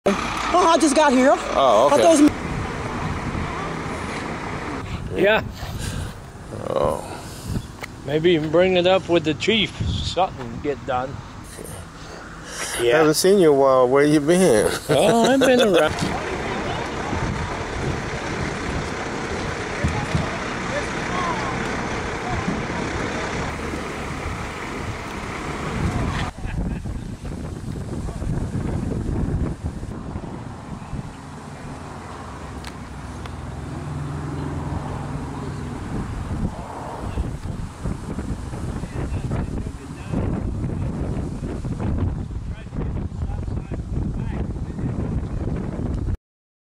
1300 block of F street Bike vs pickup
Altus Police Department and jackson county  EMS responded to a bicycle vs pickup truck accident,  a teenager headed northbound on Loyld Street entered  the intersection of F street in the 1300 block and collided with a  westbound pickup truck on the driver side front fender,  the teenager was wearing headphones and according to witnesses  was not paying attention as he cross the intersection. the teenager complained of back pain and additional injuries he was treated by Jackson County EMS the full extent of his injuries are unknown at this time